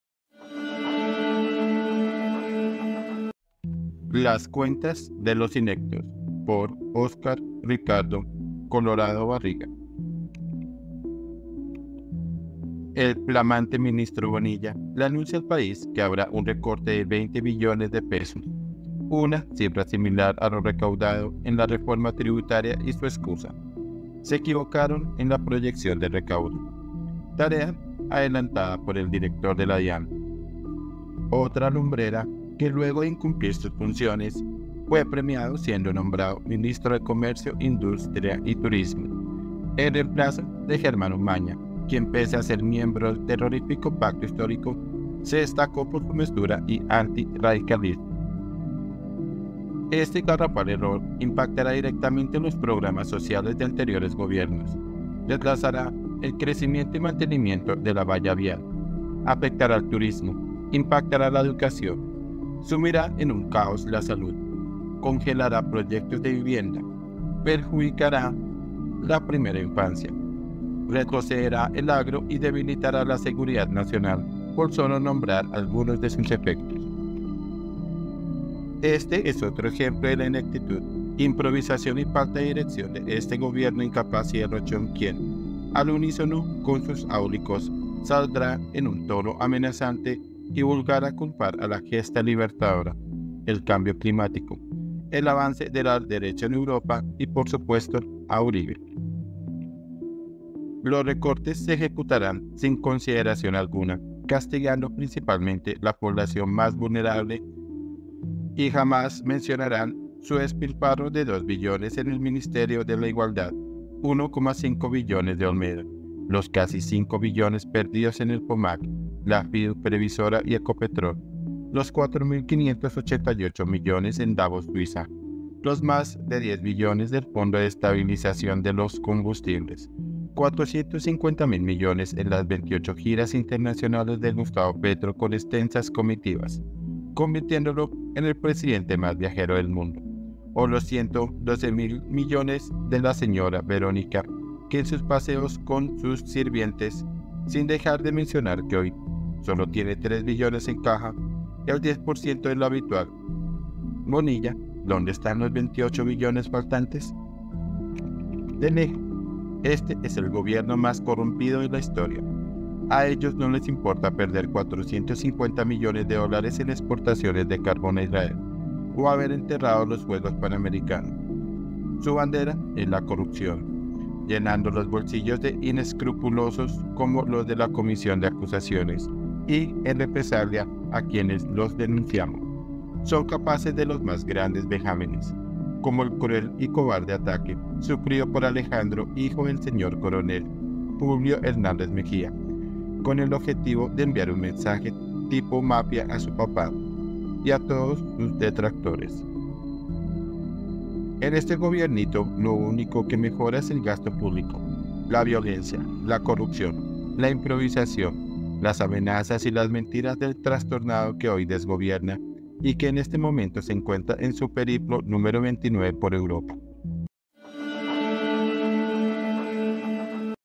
LAS CUENTAS DE LOS INEPTOS 🔴 Por: Óscar Ricardo Colorado Barriga
El flamante ministro Bonilla, le anuncia al país que habrá un recorte de 20 billones de pesos, una cifra similar a lo recaudado en la reforma tributaria y su excusa: ” se equivocaron en la proyección del recaudo”, tarea adelantada por el Director de la DIAN, otra lumbrera que luego de incumplir sus funciones, fue premiado siendo nombrado Ministro de Comercio, Industria y Turismo en reemplazo de Germán Umaña, quien pese a ser miembro del terrorífico Pacto Histórico, se destacó por su mesura y anti radicalismo.
columna ÓSCAR RICARDO COLORADO BARRIGA